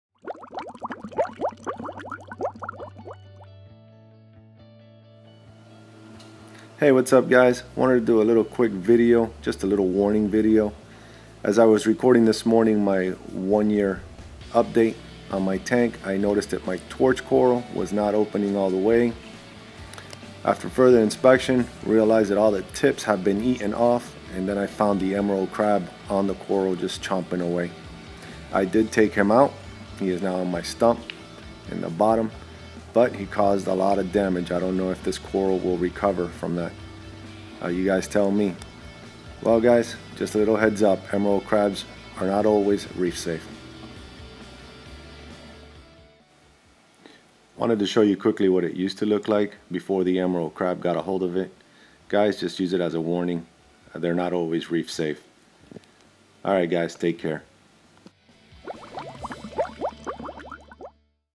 120g Reef Tank - Emerald Crab - Not Always reef Safe
Discovered my Emerald Crab eating my coral this morning, I am sure you guys can tell in my voice how upset I was that a $2 Crab ate my $150 coral. Take it as a warning guys, the Crab was only in the tank for 3 days.